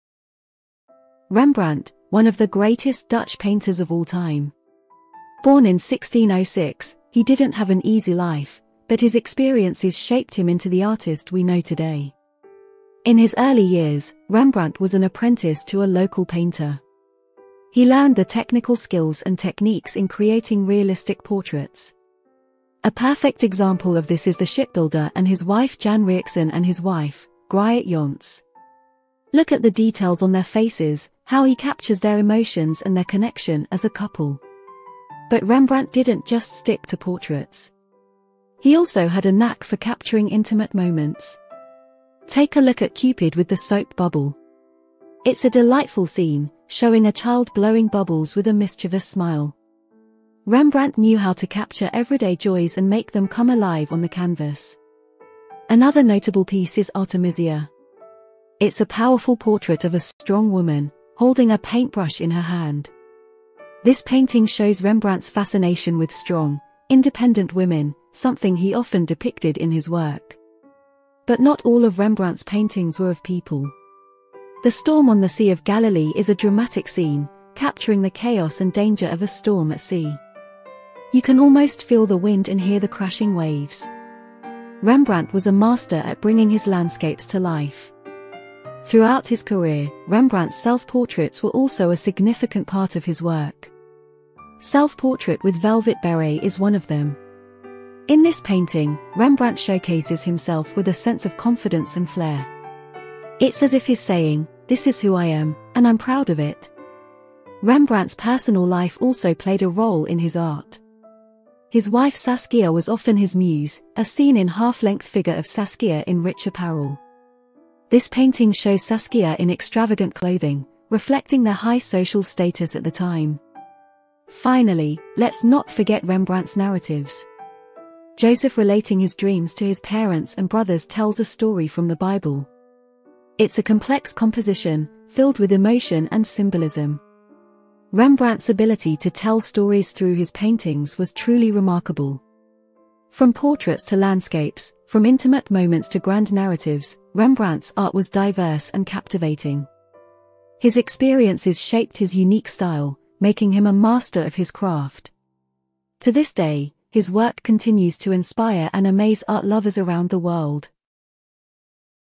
Rembrandt: A collection of 10 oil paintings with title and year, 1633-1634 [4K]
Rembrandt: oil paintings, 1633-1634
The Shipbuilder And His Wife Jan Rijcksen (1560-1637) And His Wife, Griet Jans, 1633
Cupid With The Soap Bubble, 1634
Artemisia, 1634
Scholar At His Study, 1634
Young Man With A Sword, 1633
Portrait Of An Old Man, 1634
Self-Portrait With Velvet Beret, 1634
The Storm On The Sea Of Galilee, 1633
Half-Length Figure Of Saskia In Rich Apparel, 1633-34
Joseph Relating His Dreams To His Parents And Brothers, 1633
OnlineArtGallery,CollectionOfPaintings,ArtHistory,RembrandtHarmenszoonvanRijn